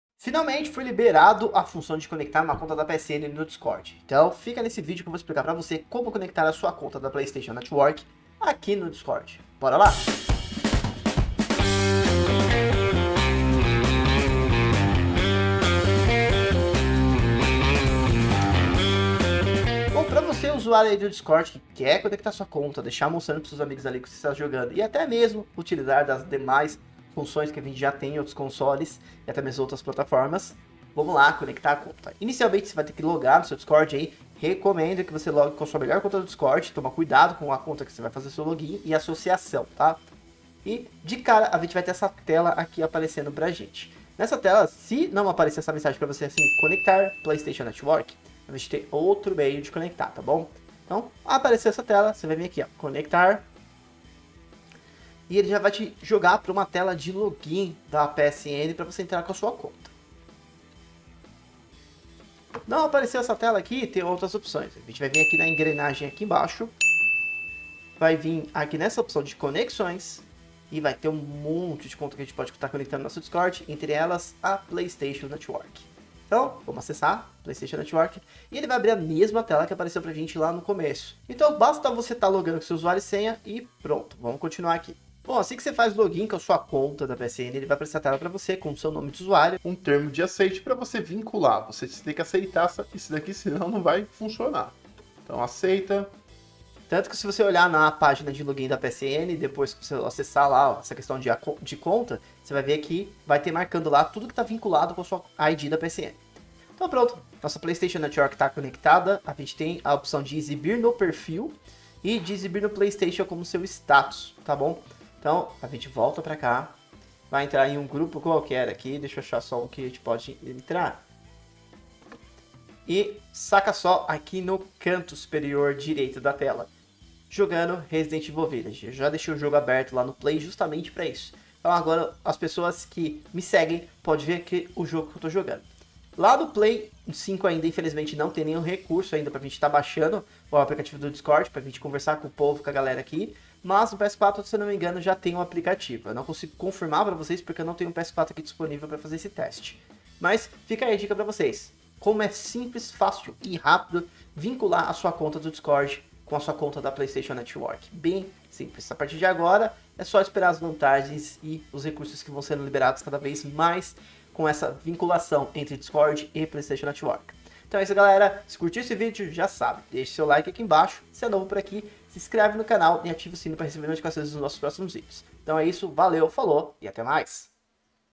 VINCULANDO SUA PSN ID NO DISCORD
Finalmente ficou disponível vincular uma conta PSN com o Discord, então dá o play no vídeo para entender como fazer.

ALGUM LINK QUEBRADO? POR FAVOR NOS AVISE NOS COMENTÁRIOS =D